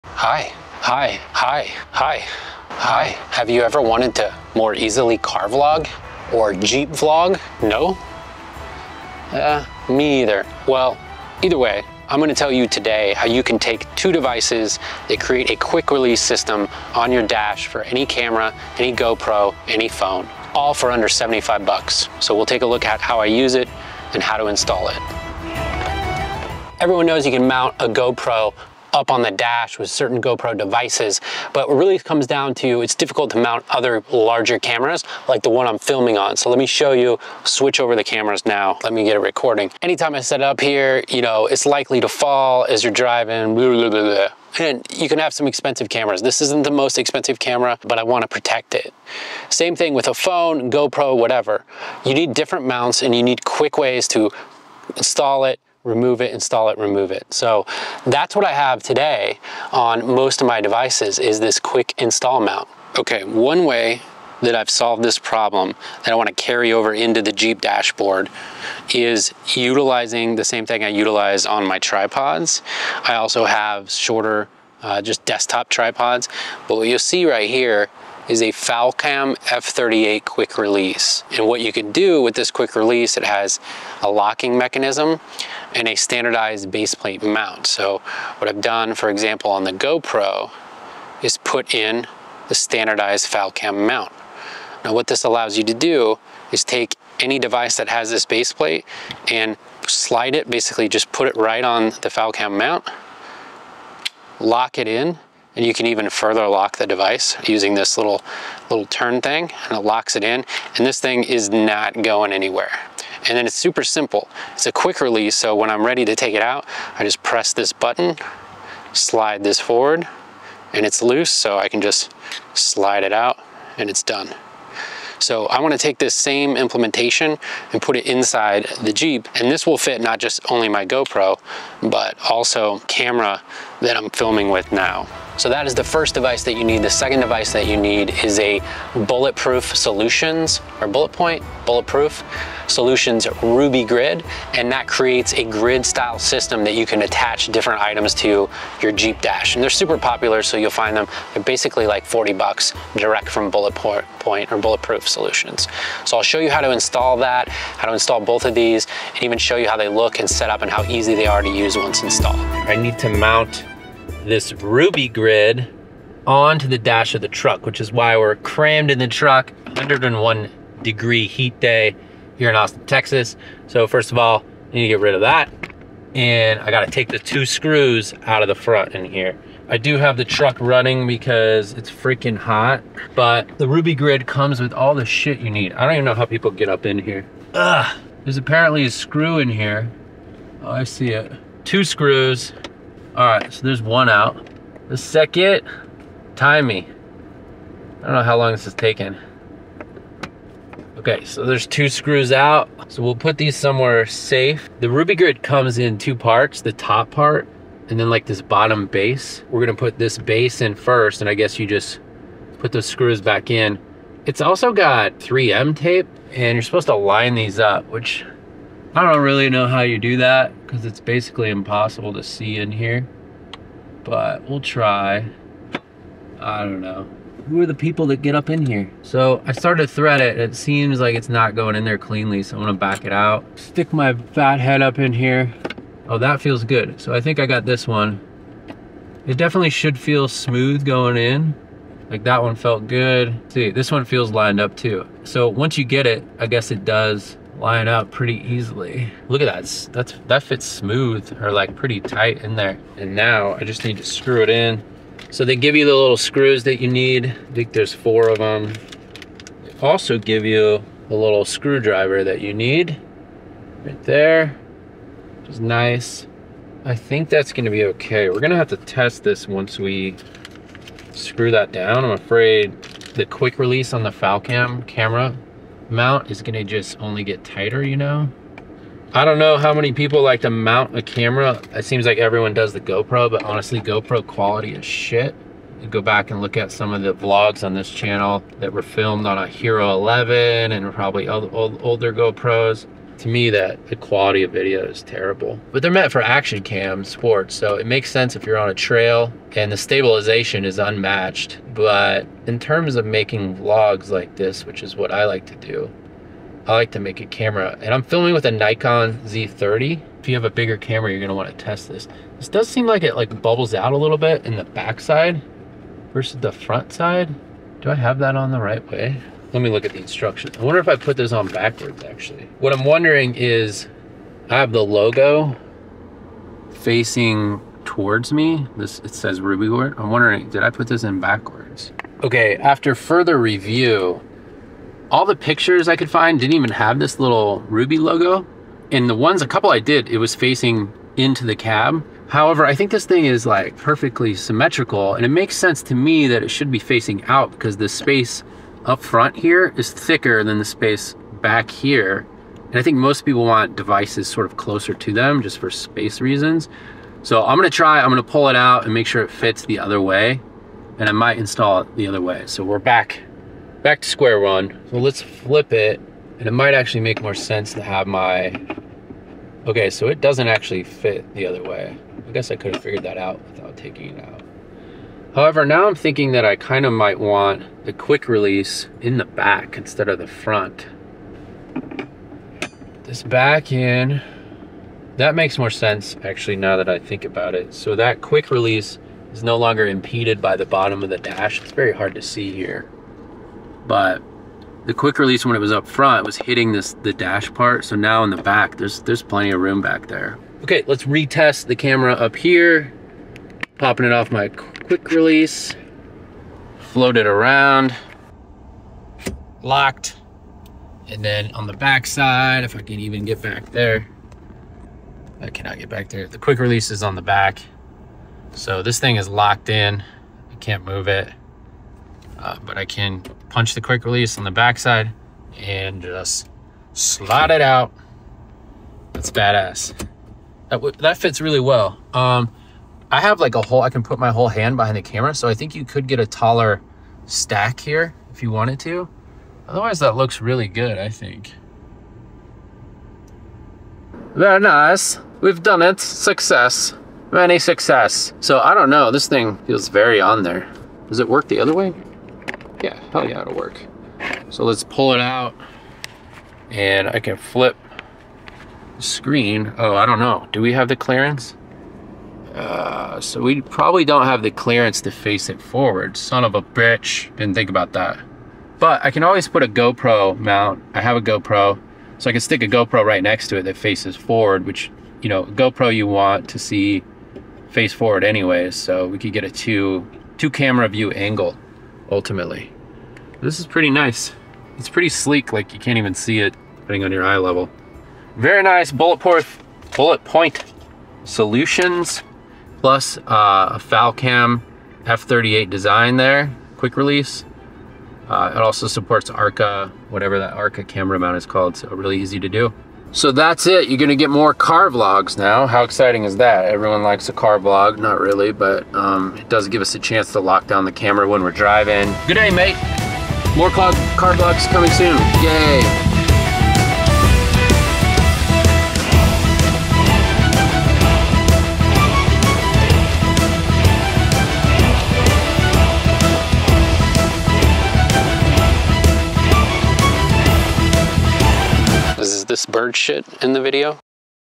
The BEST Jeep Gladiator GoPro Mount | Quick Release!
I've been searching for the best way to mount not only a camera but also a GoPro and my iPhone and I think I have found it! Check out how I use two devices to create a quick release mounting system on the dashboard of my Jeep Gladiator.

Products used are from the following companies.